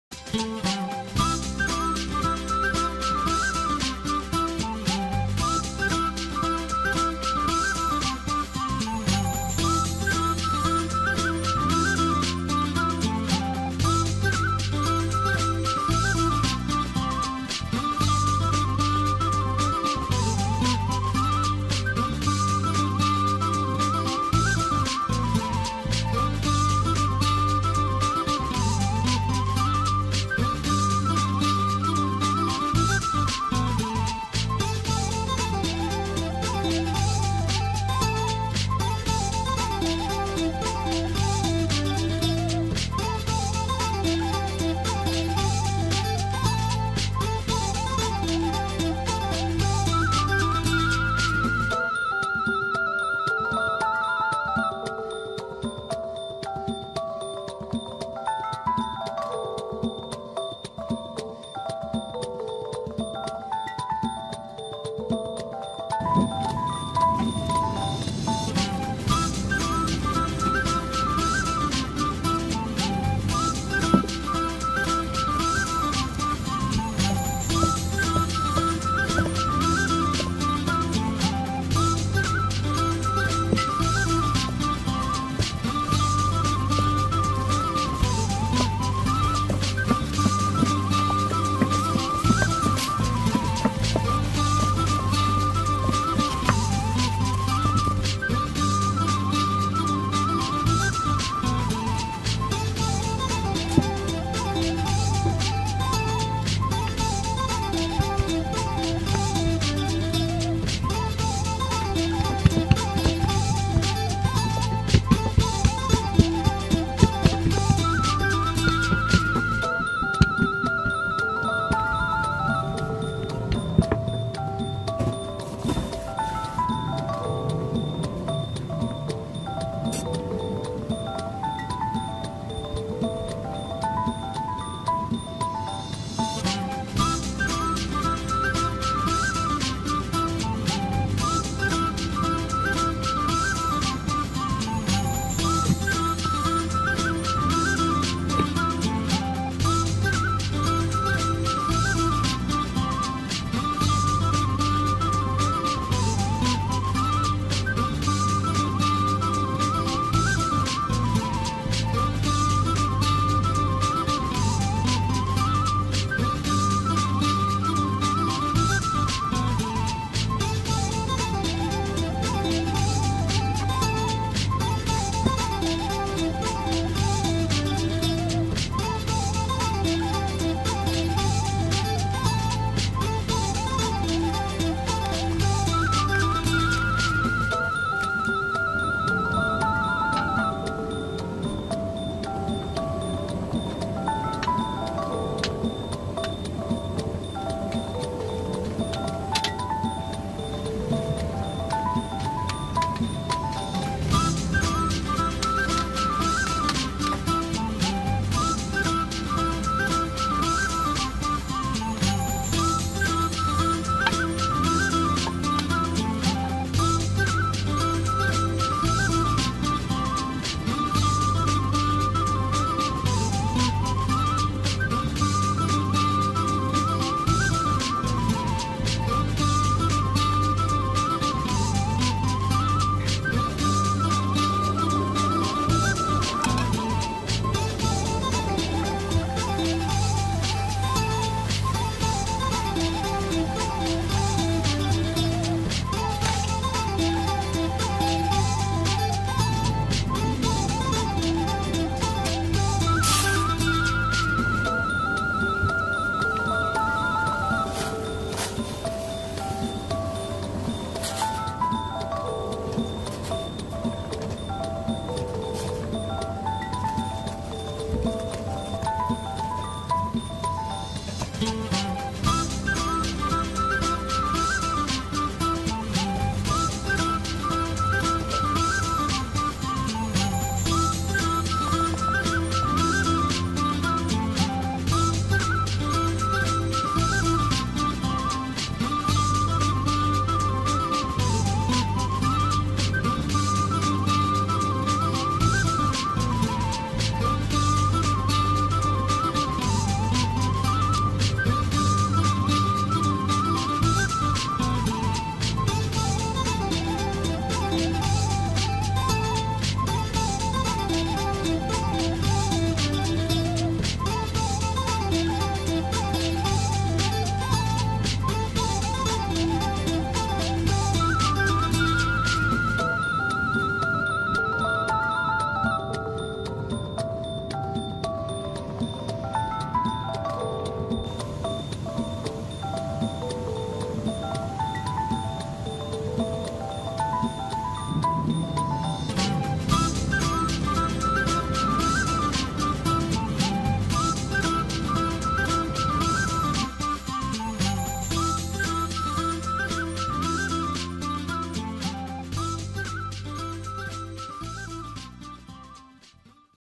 1016 ขนมโพรงแสม ขนมไทยโบราณ หาทานยาก แป้งหอมกรอบ ราดโตนดเคี่ยวหอมๆ Phong Samae dessert(Thai Dessert)
Thai cuisine
Street food